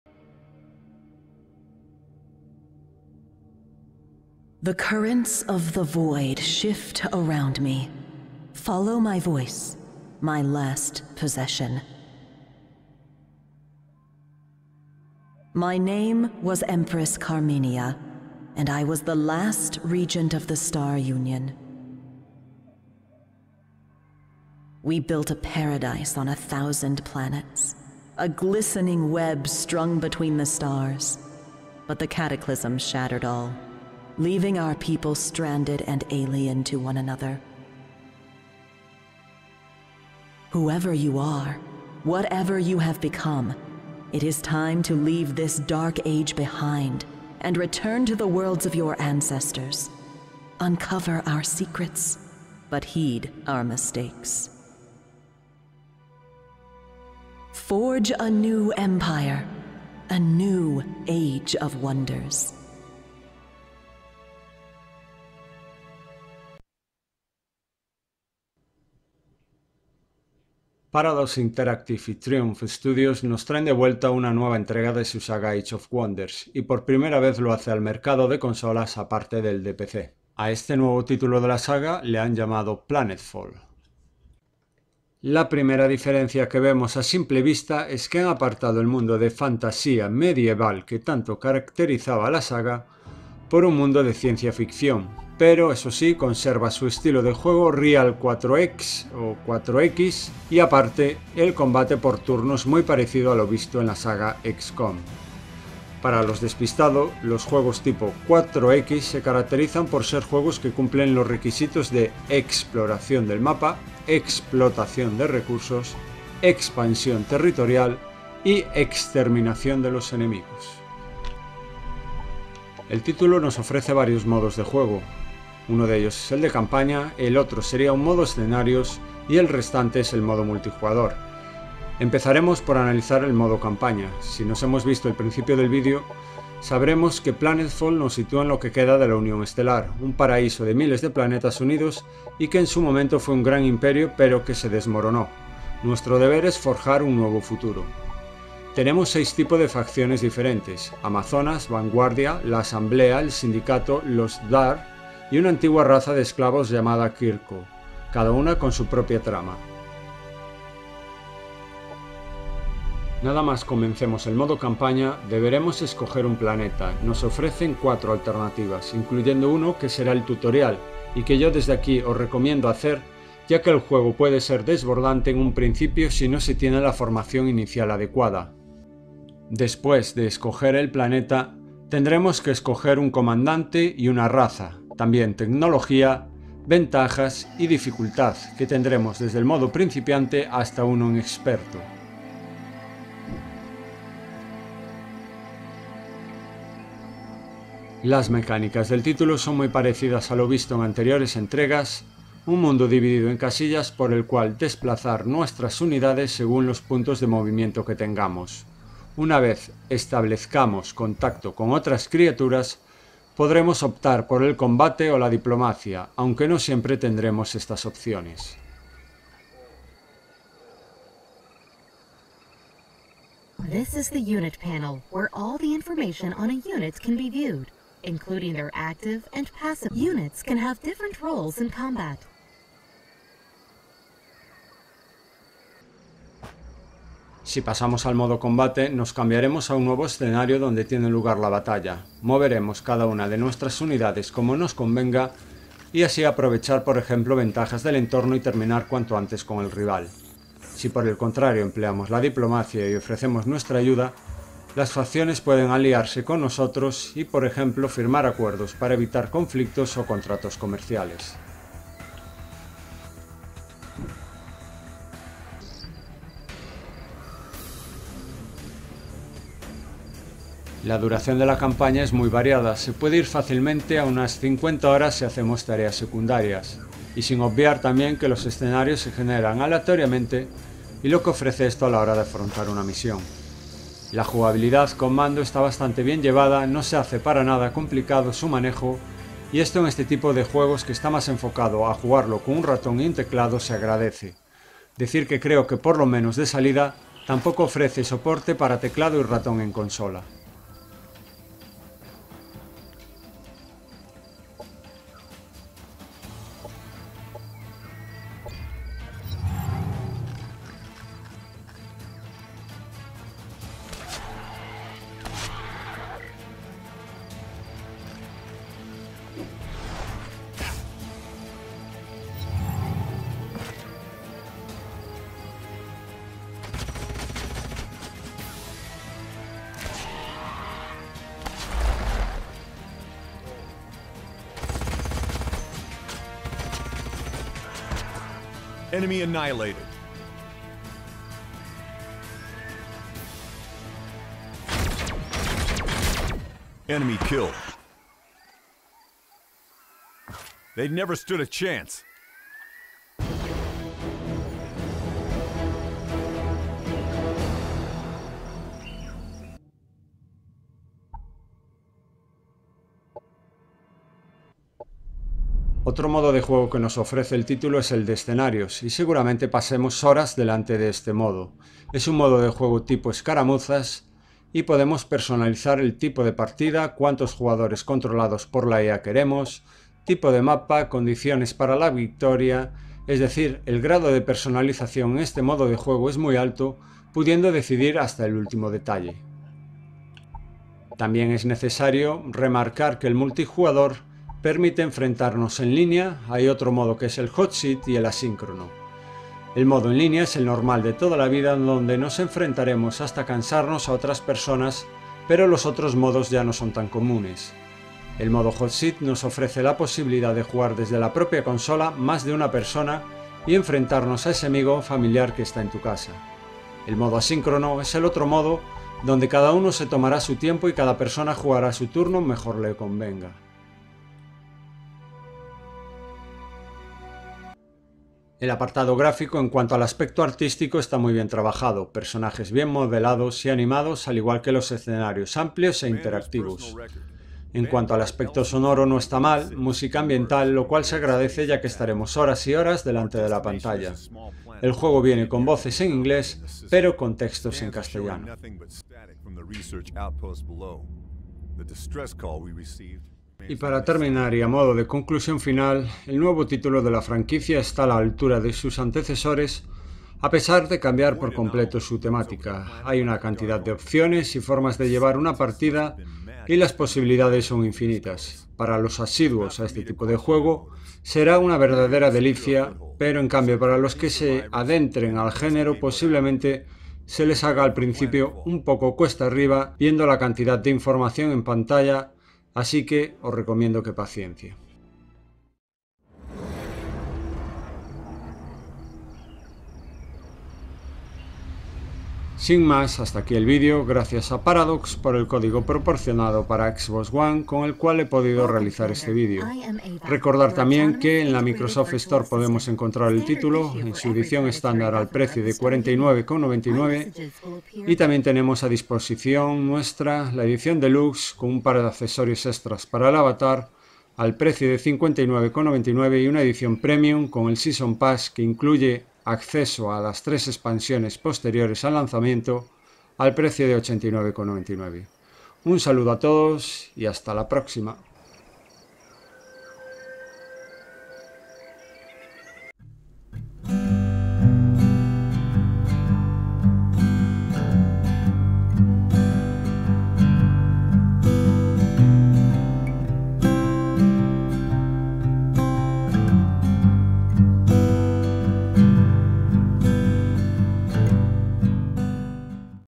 Age of Wonders: Planetfall - Analisis/Review (Xbox One)
Age of Wonders: Planetfall es el nuevo juego de estrategia de Triumph Studios, creadores de las aclamadas series Age of Wonders y Overlord:

Titulo: Age of Wonders Planetfall
Desarrolladora: Triumph Studios